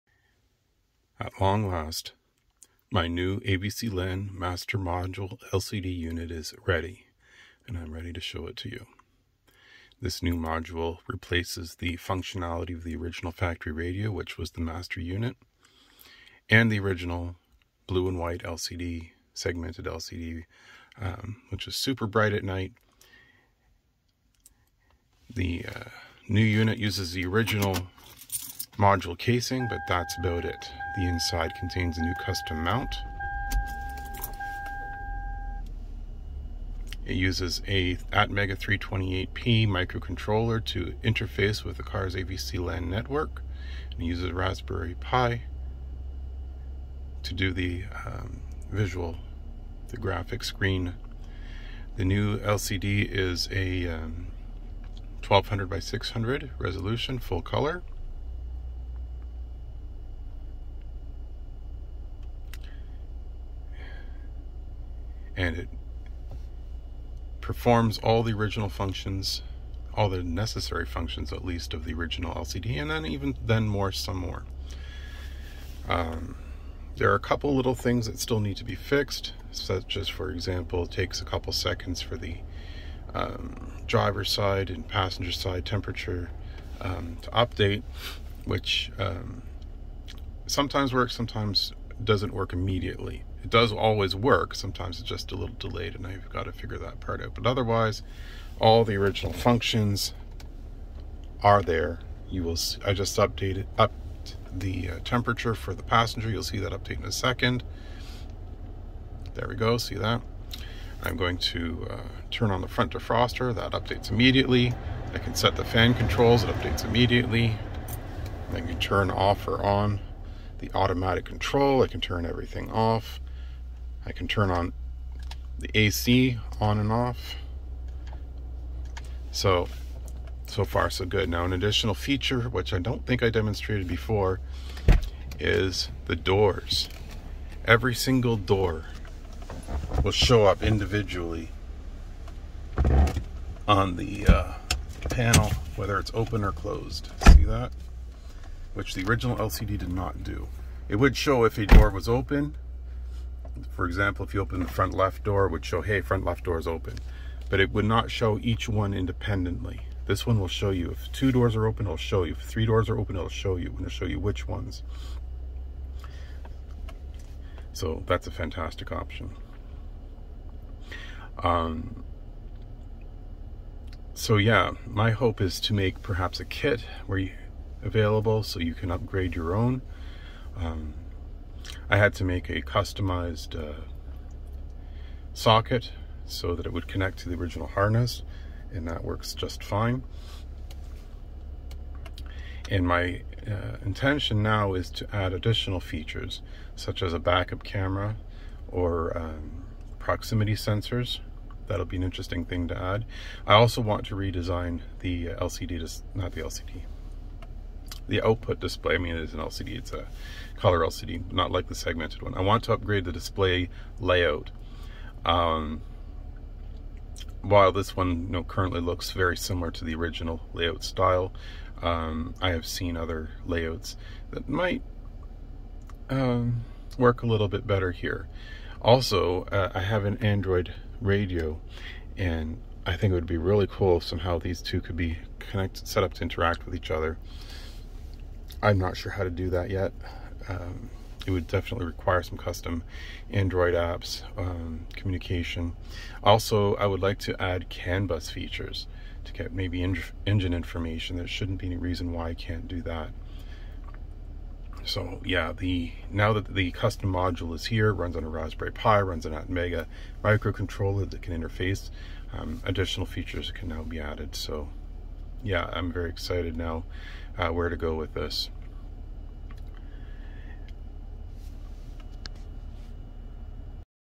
It's ready!! - New Toyota Avalon Master AVCLAN LCD Module
I have been working on this project for at least a couple years now and finally have completed the harness adapter to plug it in!  My new custom AVCLAN master LCD module replaces the master AVCLAN functionality of the factory radio so that it no longer is required to keep the factory radio in the trunk of the car!

Future plans:
1.  Implement I2C communication instead of serial
2.  Add proximity sensors
3.  Redesign the graphic layout
4.  CANBUS
5.  Backup camera?
6..  API?

Watch my previous videos to see how I got here.

Suggestions?  Please comment!